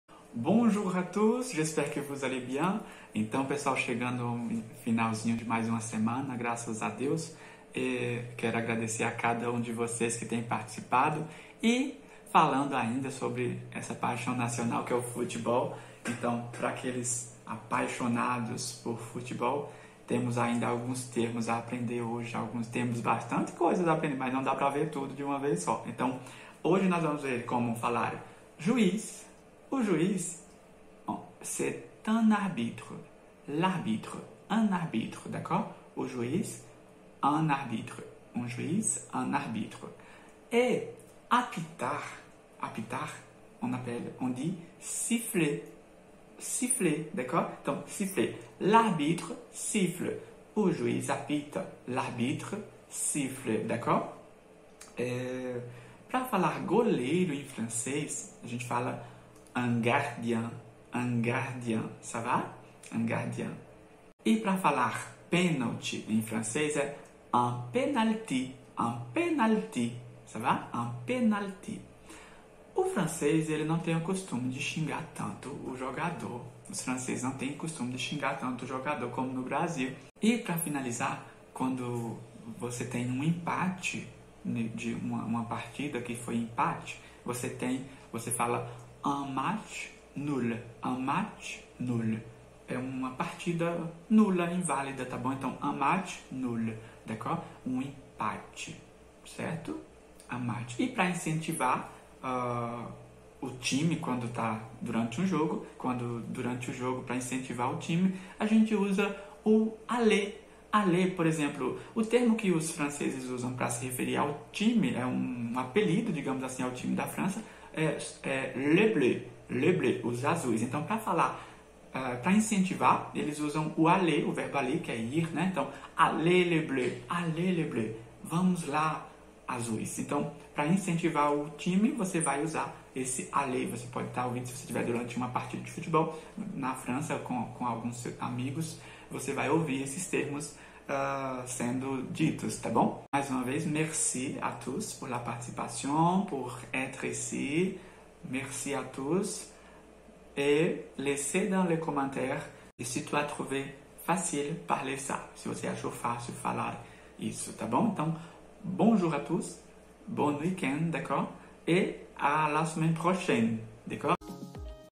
Veja como falar: juiz, goleiro, apitar...
Vamos falar de futebol?

Siffler
Apitar

Un arbitre - l'arbitre siffle
Um juiz -o juiz apita

Un gardien
Um goleiro

Obrigado minha amiga Lucineia do @cliquedocumental pela sugestão do tema 🙏🏽

Então pratique e me conta aqui nos comentários se foi fácil a pronúncia e me conta qual seu time do coração?💬👇 e manda para um(a)
amigo(a)✈️ praticar com a gente!